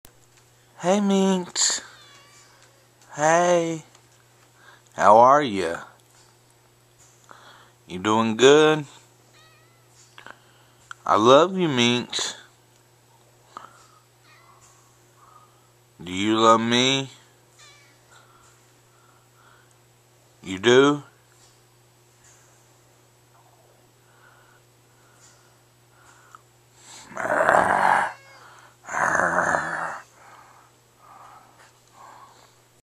Copy of cat gets scared
my cat was laying on the floor so i decided to talk to her after everything i asked her she meowed but i dont think you can here it in this video